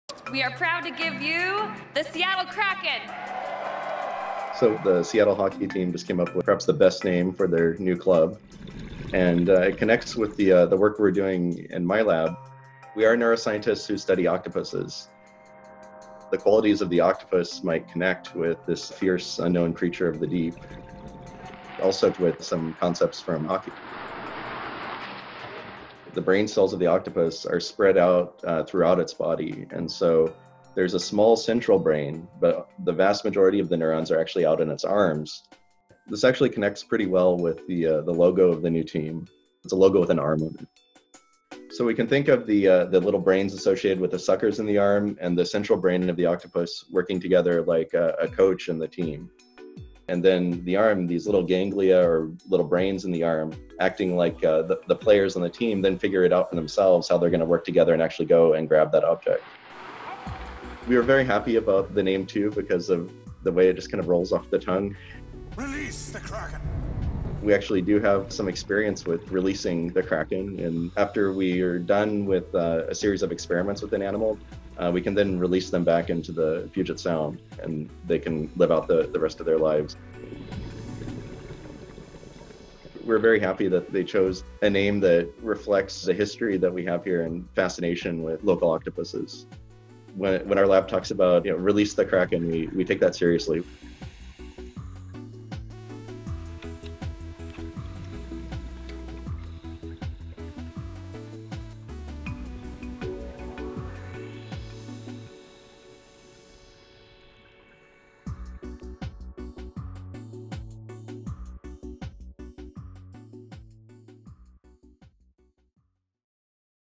UW octopus scientist on the Seattle Kraken
Seattle’s new hockey team is named for a legendary creature of the sea, and that’s a perfect fit, according to octopus researchers at the University of Washington.

David Gire, an assistant professor of psychology, studies the neuroscience of the octopus. He says their tentacles act like little brains as their arms forage for food, that one species is big and strong enough to take down a shark, and how, even though octopus are solitary creatures, they learn from each other and work together.

That’s part of what makes the Kraken such an appropriate name, and logo, for a hockey team, Gire says. With the octopus, “you’re seeing the epitome of teamwork. You’re seeing an arm with a bunch of little brains, and they’re all working together to guide where that arm goes.”

Gire's team has had many an opportunity to 'release the Kraken' when they return an octopus to the Puget Sound when they are done observing it in the lab.

This video was produced by Kiyomi Taguchi for UW News.